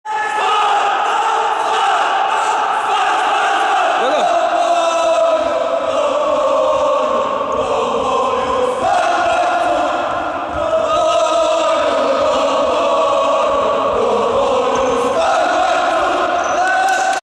SPAR FUTSAL TV | Triumf SPAR w Bać Pol CUP 2017
Mistrz Bać Pol CUP XXV edycja turnieju w futsalu.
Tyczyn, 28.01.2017